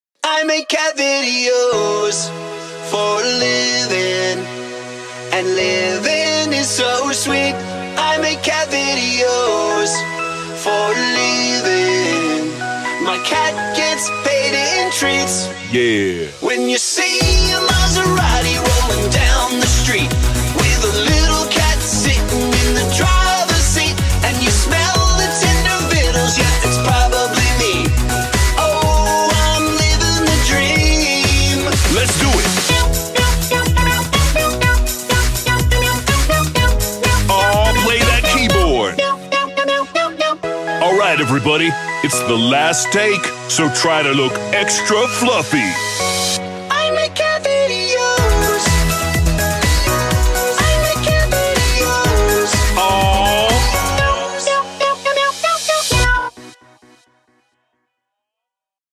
Video star | I Make Cat Videos For a Living w/Cutie&Pebbles
I Make Cat Videos For a Living
Actors: Molly, Cutie, and Pebbles!